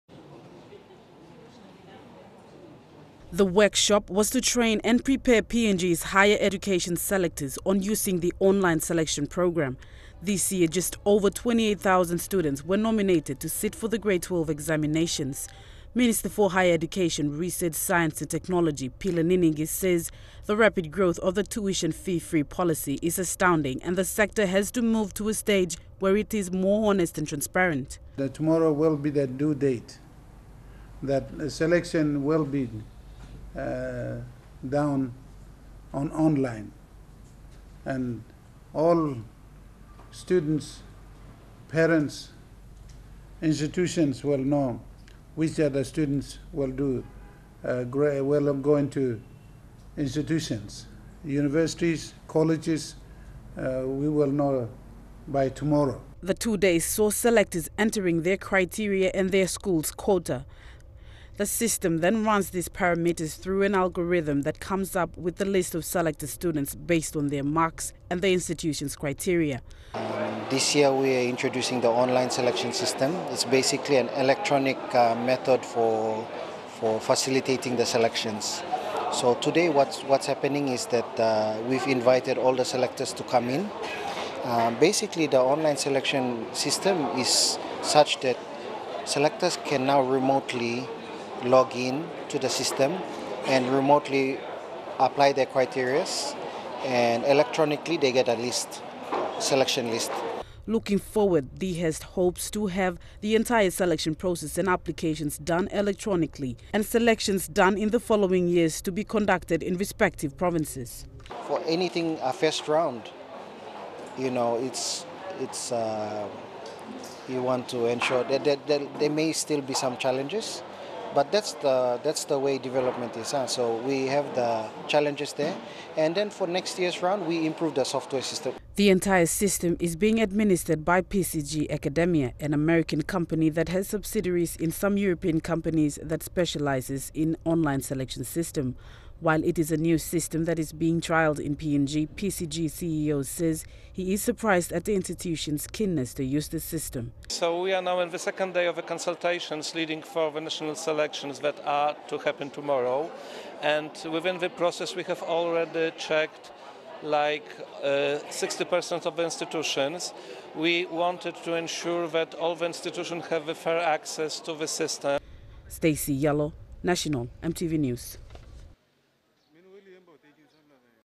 Institutions Undergo Two-day Training Before the First Online Selection Begins
Selectors from Higher Learning Institutions have undergone a two-day introduction to the Electronic Selection System that will be launched tomorrow by Higher Education Minister, Pila Niningi.

Representatives from the six major universities and other technical and business institutions under the DHERST were present.

The online selection is the first of its kind that DHERST is introducing to deal with fairness, accountability, and capacity issues during Selections.